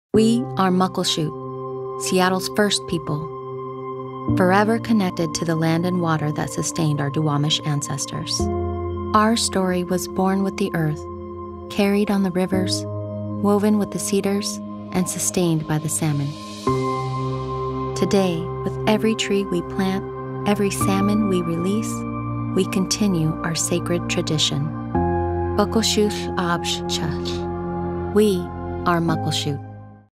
Seattle's First People | Muckleshoot Tribe
We are Muckleshoot, the first people of Seattle, forever connected to the land and water that sustained our Duwamish ancestors since time immemorial. We are caretakers of this land and of the salmon that thrive in these waters, and we believe in doing everything we can to protect the Puget Sound region.

Today, we plant nearly a half million trees and raise and release more than 10 million salmon each year.

Investing to preserve our culture, and the land, water, and wildlife that protects us all